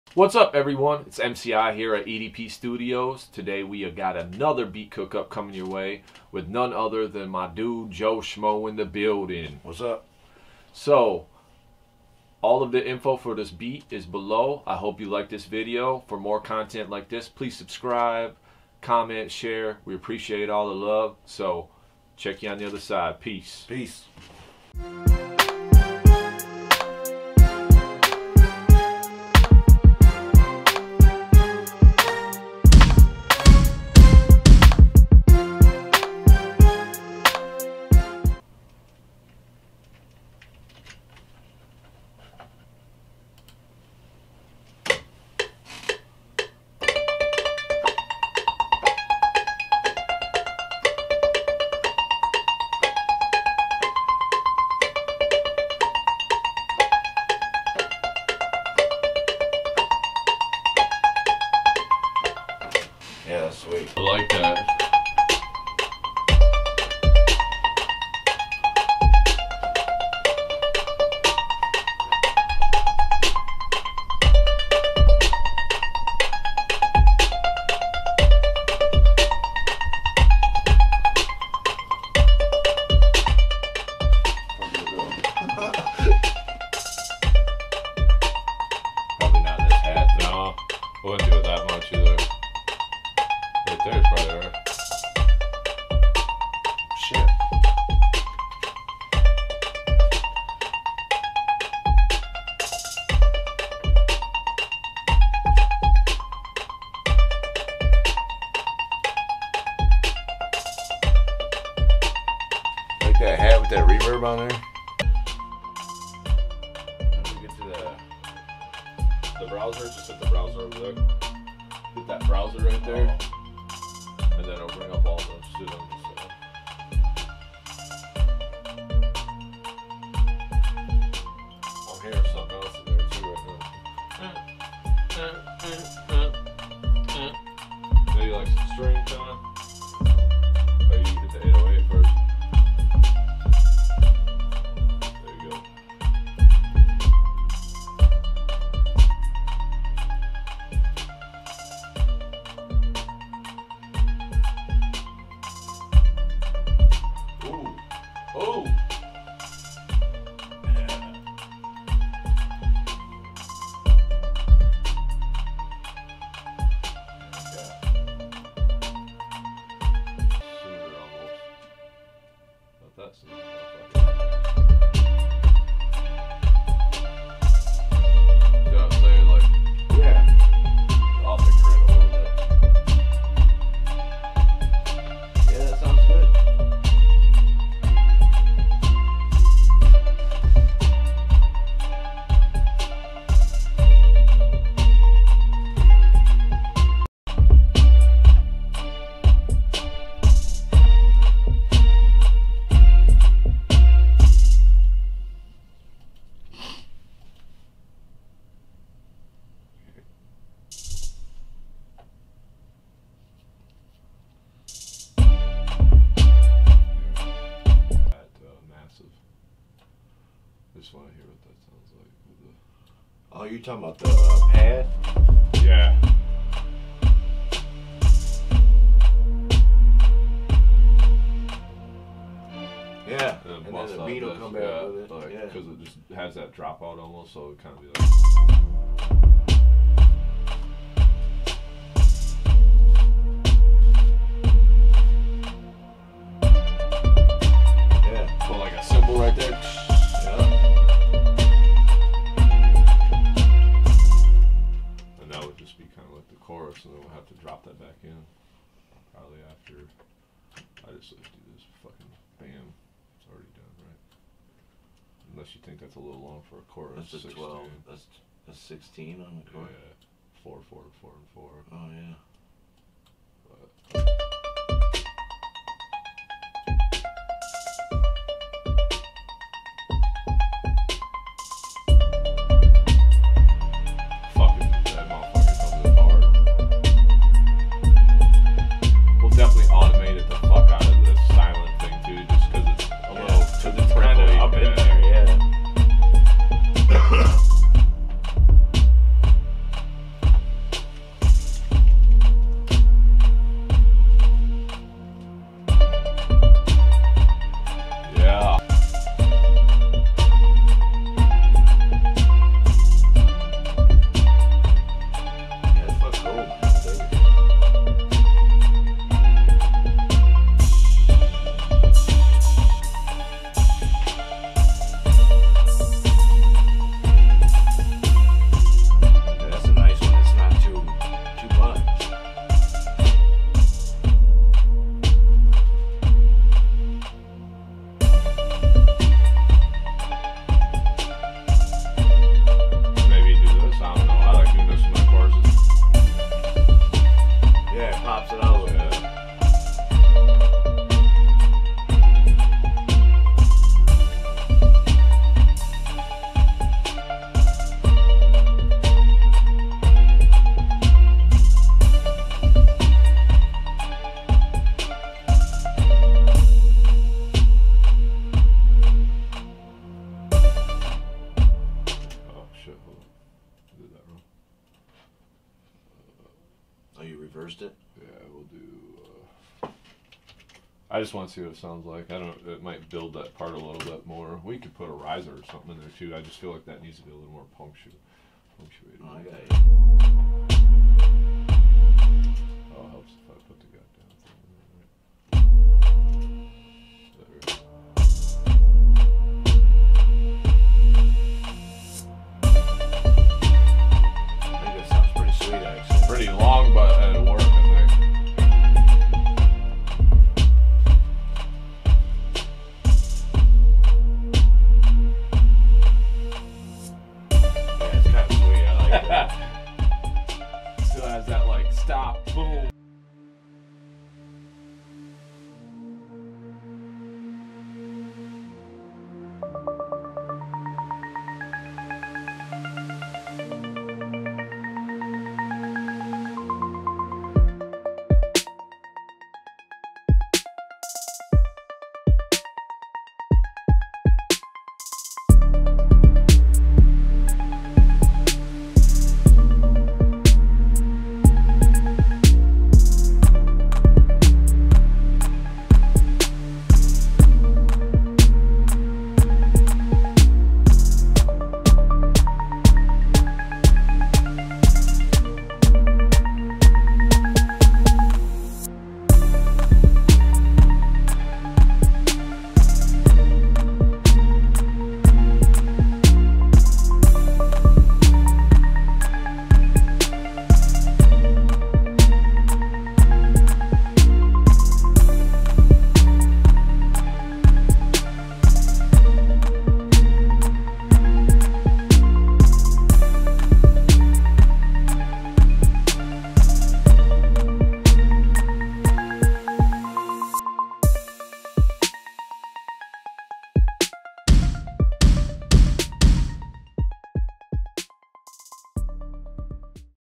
E.D.P. Beat Cook up Episode 2 with Joe Schmo 2017| Melodic Trap type beat
Get a look inside a beat making session with M.C.I. and Joe Schmo at E.D.P. studios. November 2017 Melodic Trap cook up.

Produced by